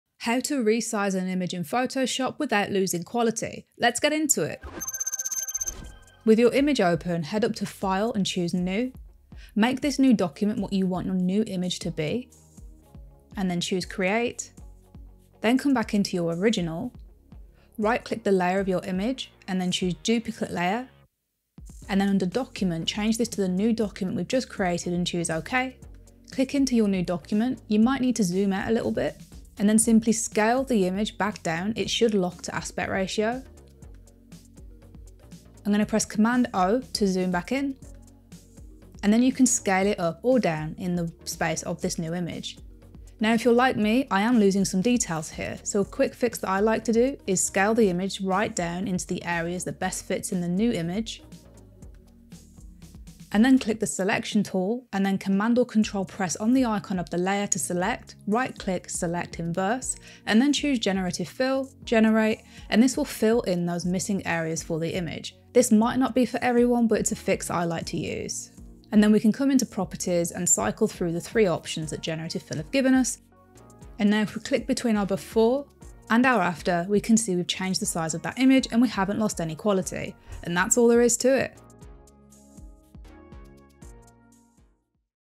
How To Resize An Image in Photoshop Without Losing Quality
Learn how to resize an image in Photoshop without losing quality. This Photoshop tutorial shows a simple technique that will make your image suitable for any project type.